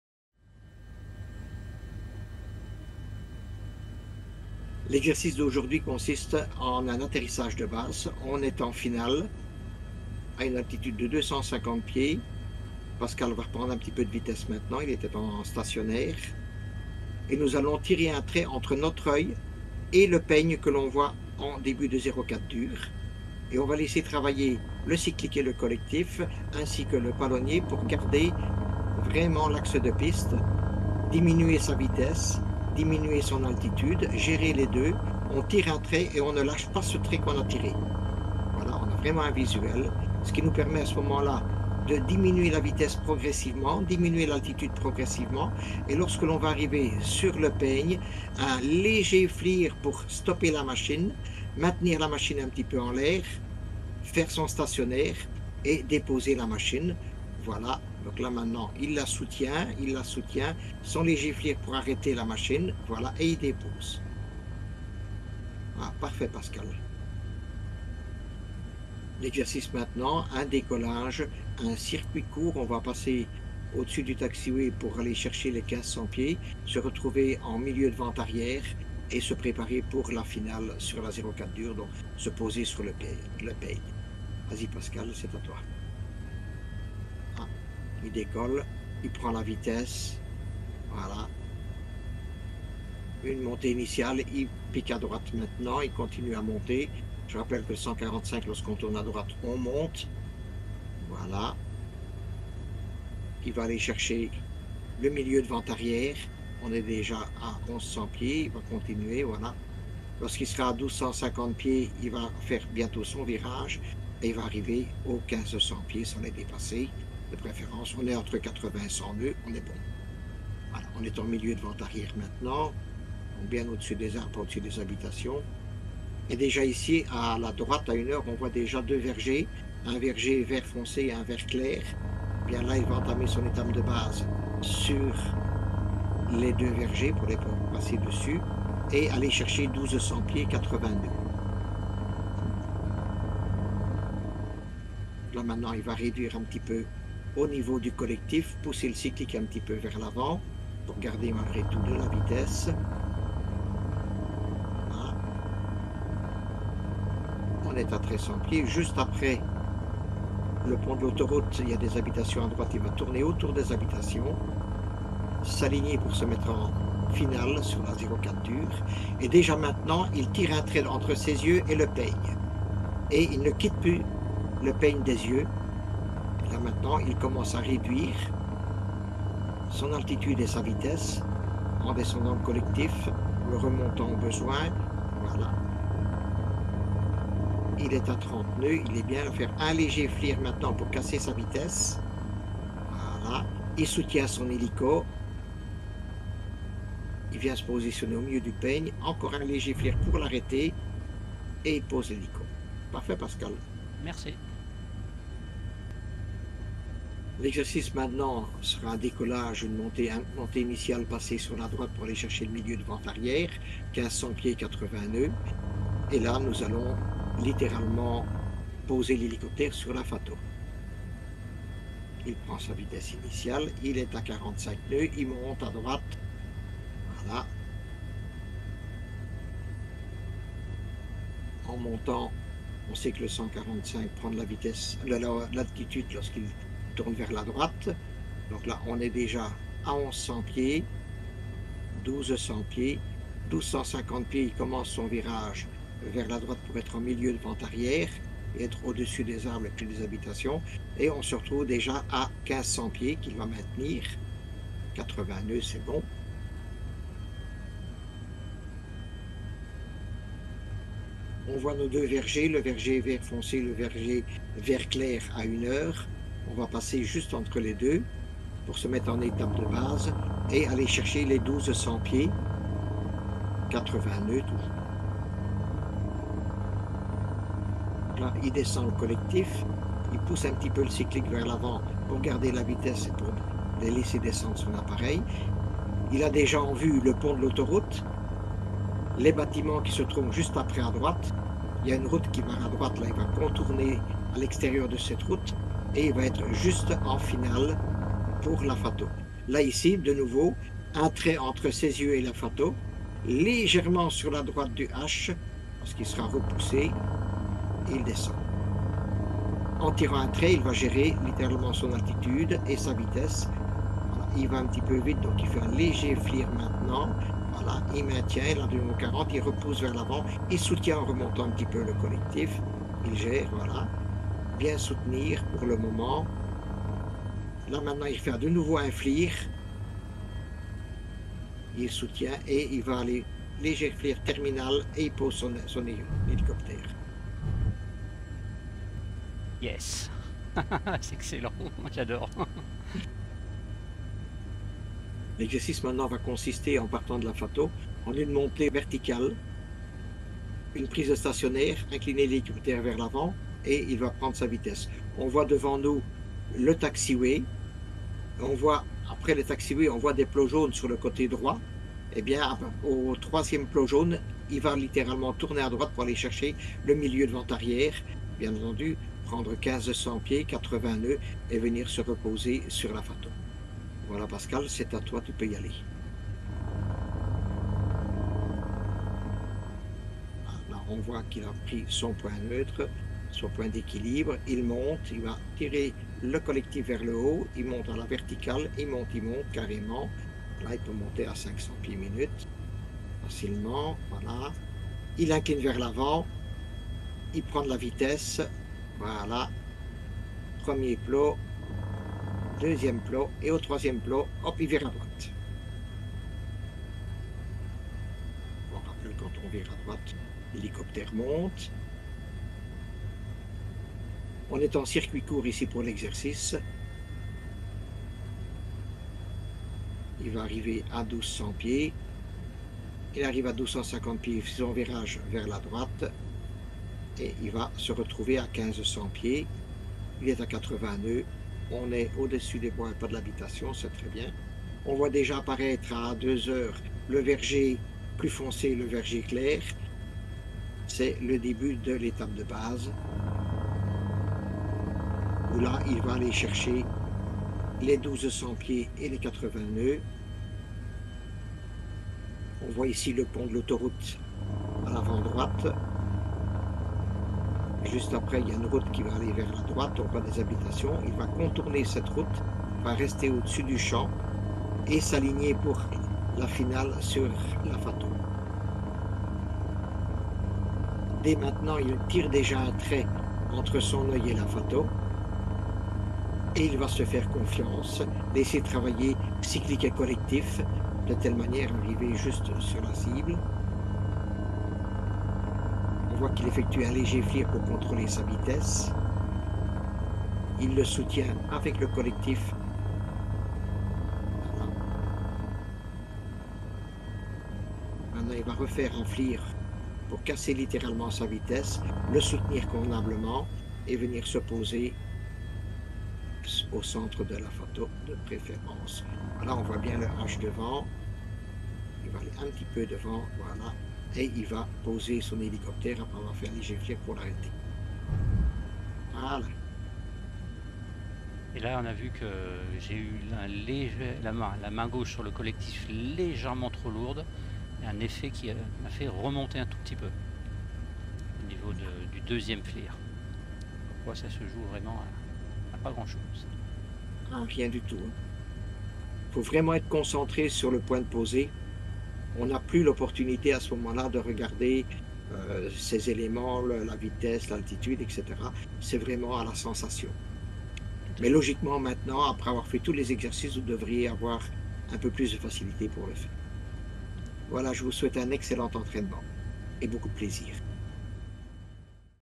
Pilotage hélico #16 Atterrissage, FATO et tour de piste - mardi 04/04/2023
Nous sommes sur la fin de cette série, il s'agit à présent de mettre en pratique tous ce qui a été vu : quoi de mieux qu'un tour de piste ?
Retour sur l'atterrissage avant de partir sur un circuit court pour commencer et pour savoir comment travailler rapidement la séquence décollage - montée - palier bref - descente et atterrissage. Dis autrement un maximum d'actions en un minimum de temps !
Attention, il ne s'agit pas de faire la course, c'est pour constater que cet exercice est dense en mania...
Pensez à vous auto-évaluer avec Little Navmap et Fligth Recorder.

0:00:00 atterrissage sur la 04 dure
0:01:10 tour de piste en circuit court pour revenir sur la 04 dure
0:03:25 idem mais pour se poser sur la FATO
0:05:44 depuis la montée verticale et prise de vitesse pour revenir se poser
0:08:40 débriefing
0:09:22 FIN